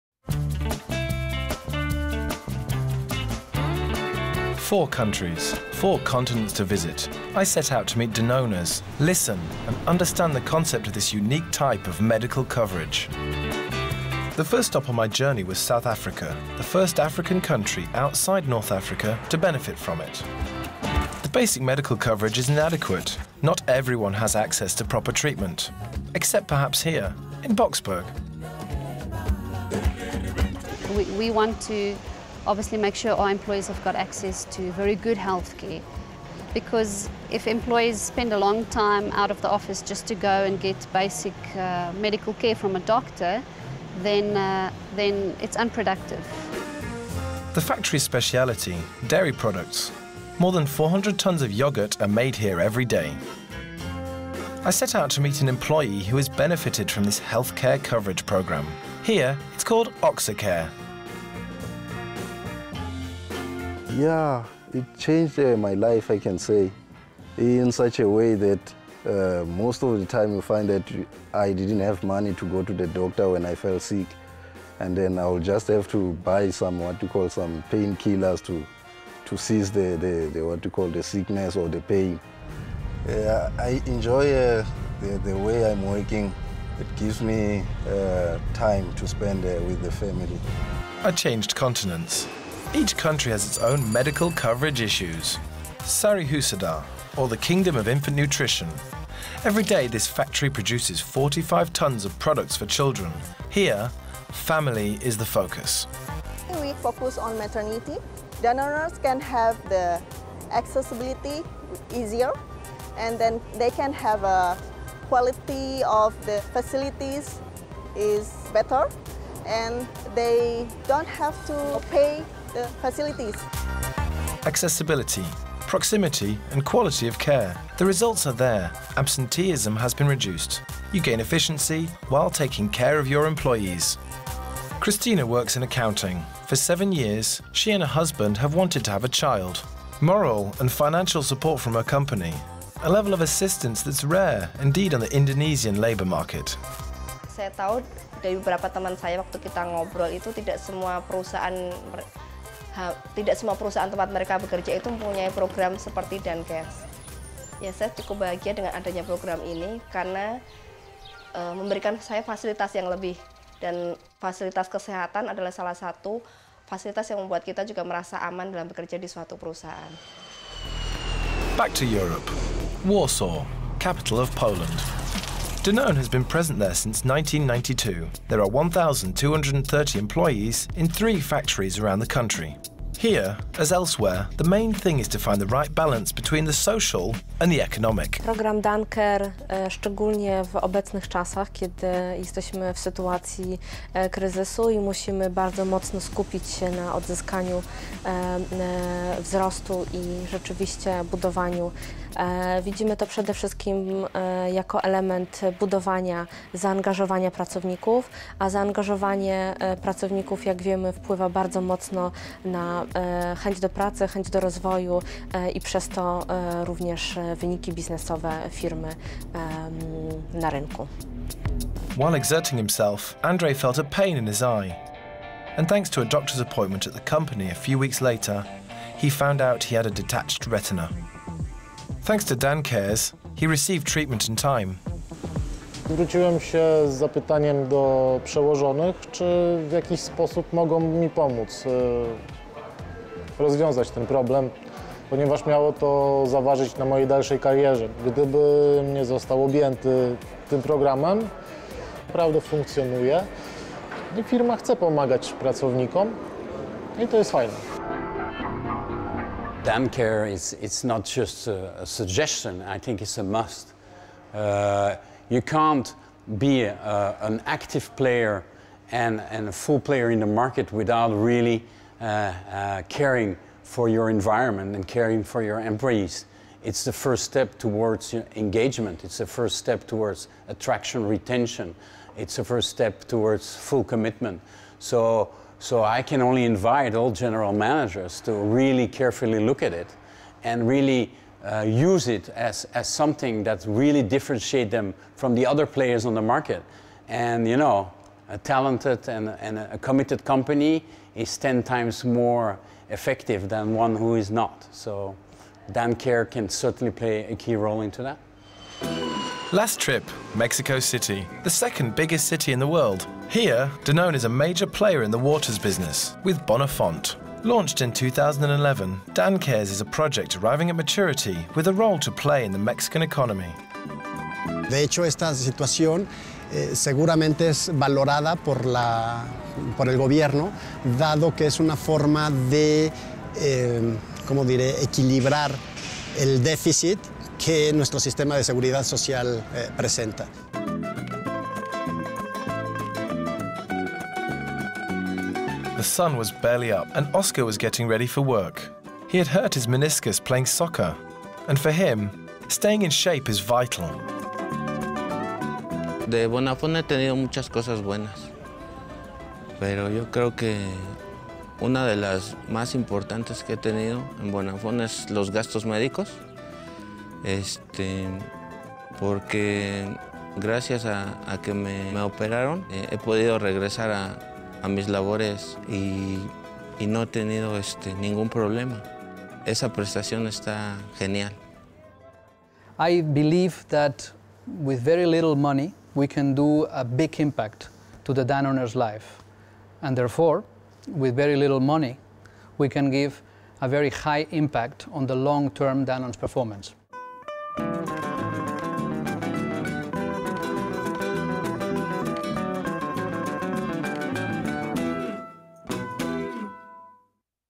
Dan'Cares Program
In 2010, Danone launched The Dan'Cares program. It aims at bringing basic medical coverage for every employee, in particular for: hospitalization, maternity and outpatient care.